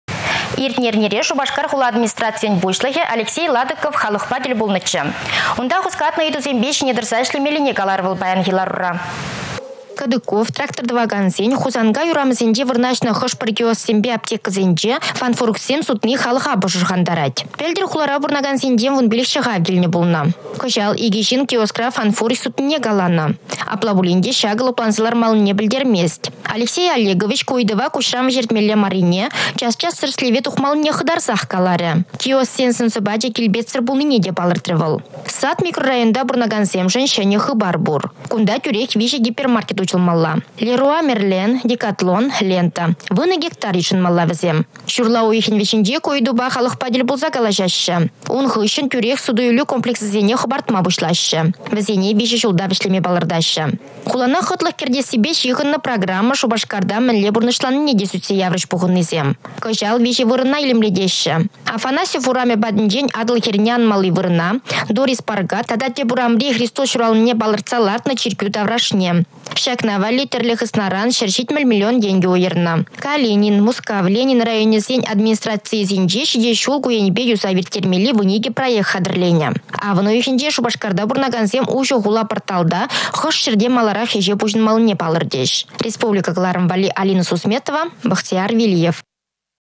Хула илемленет
Иртнĕ эрнере Шупашкар хула администрацийĕн пуçлăхĕ Алексей Ладыков халăхпа тĕл пулнăччĕ. Унта хускатнă ыйтусемпе çине тăрса ĕçлемеллине каларĕ вăл паянхи ларура.